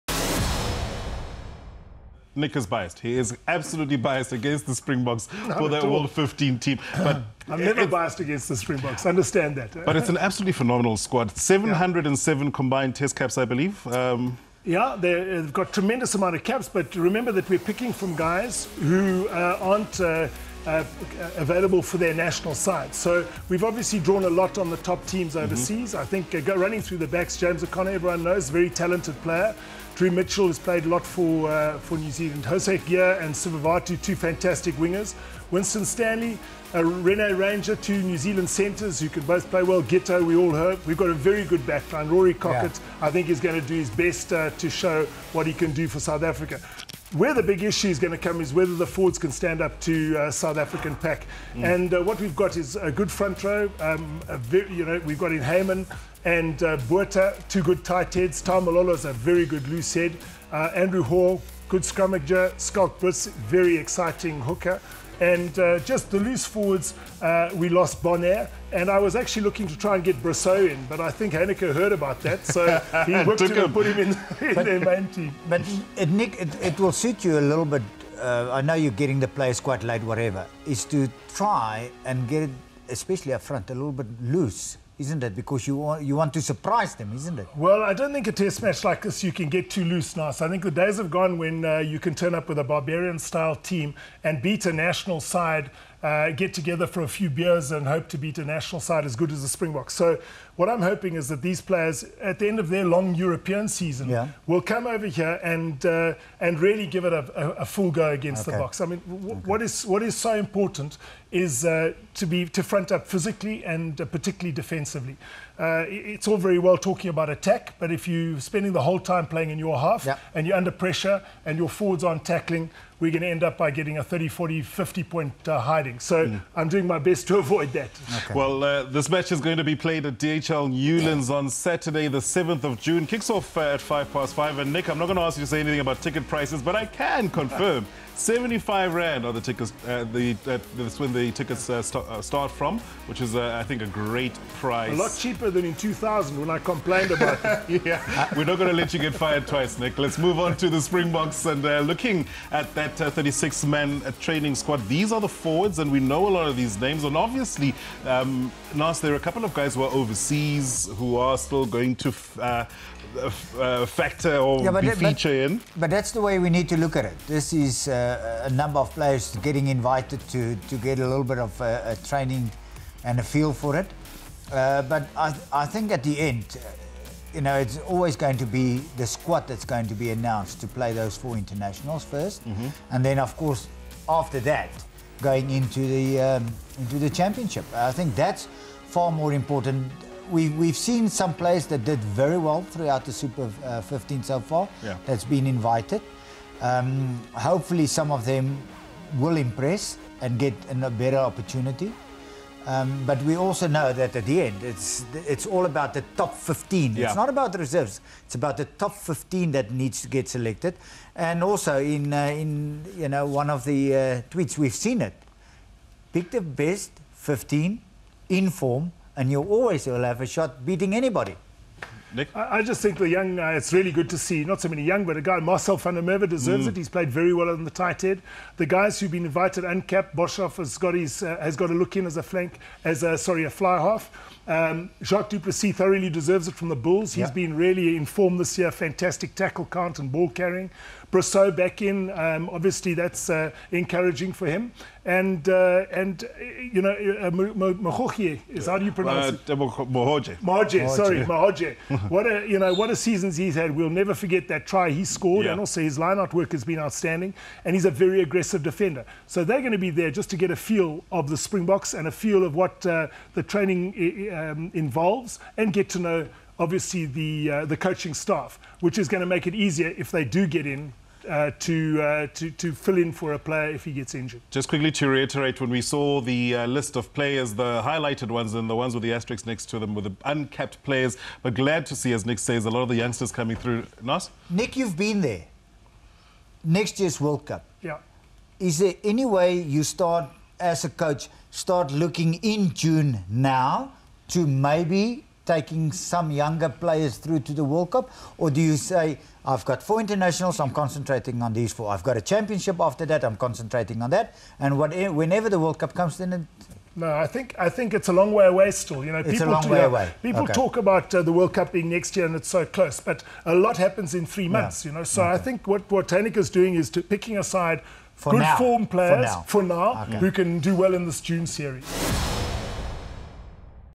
Nick Mallett discusses World XV squad
World XV coach Nick Mallett discussed his selection of the squad and gave thoughts on his opponents, the Springboks and the team's upcoming international season, with legend Naas Botha